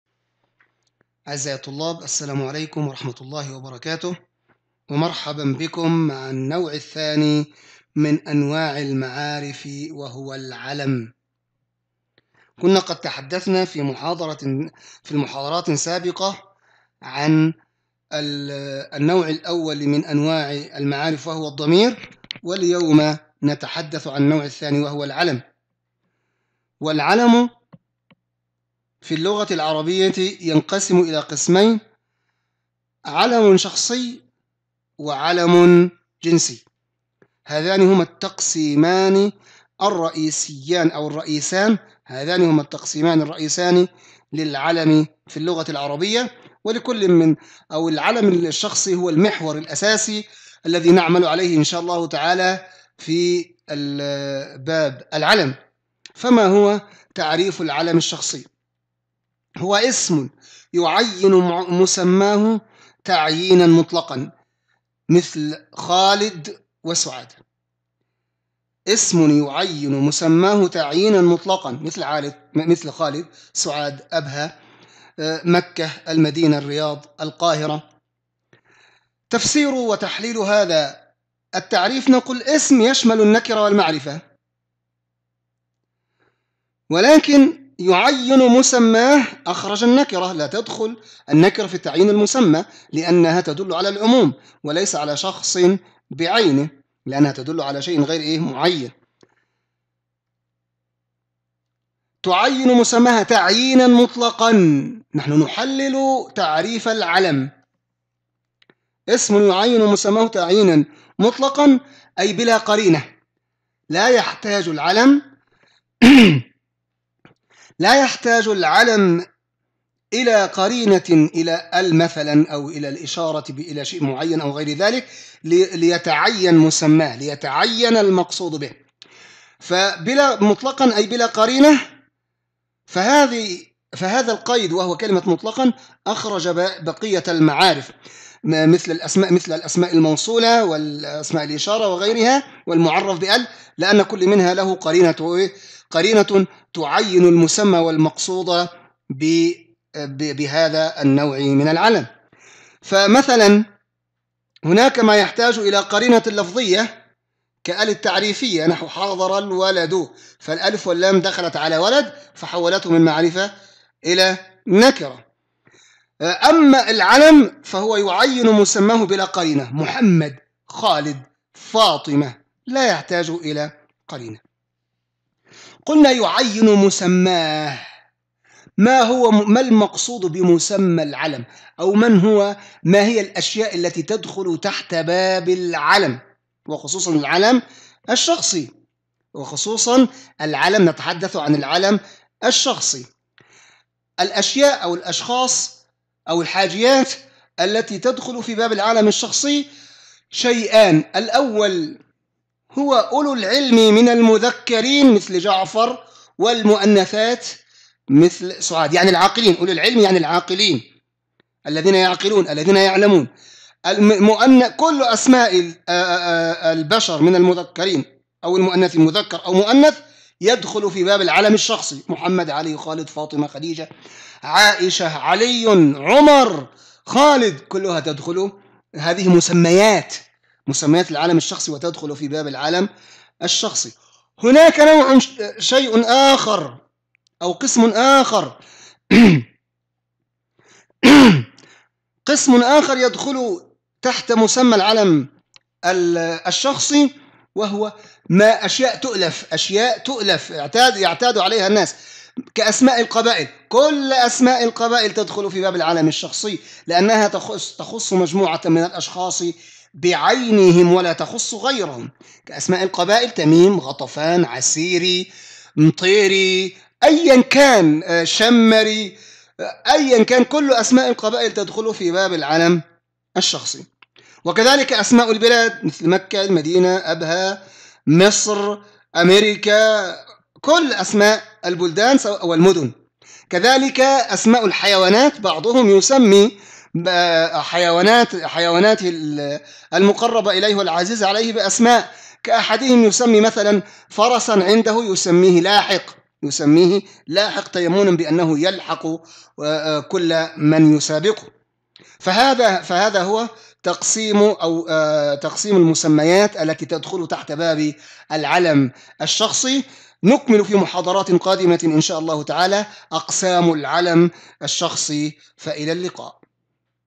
النحو العربي | 5-11 | العلم
الوحدة 5 - الدرس 11 : العلم
أقسام العلم: شخصي، وجنسي